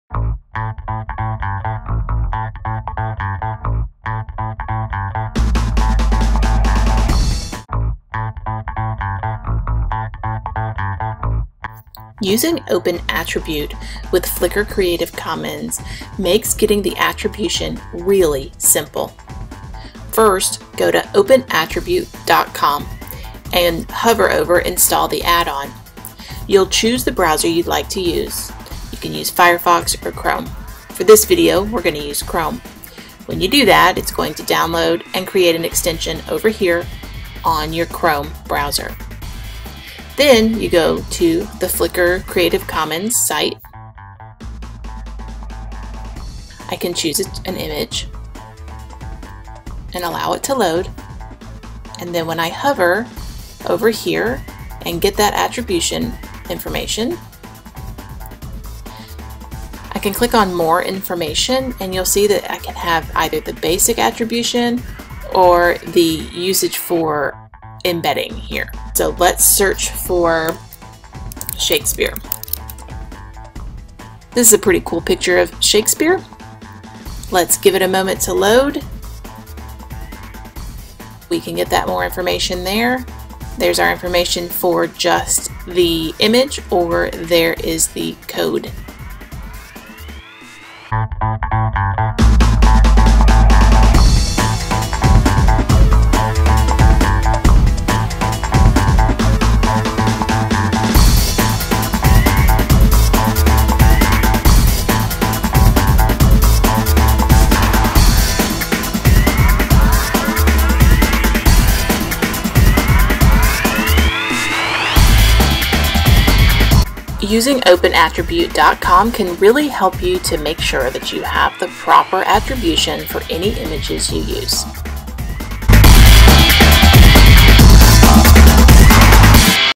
Take 5: OpenAttribute
Use the OpenAttribute add-on with FlickrCC to be sure you have proper attribution.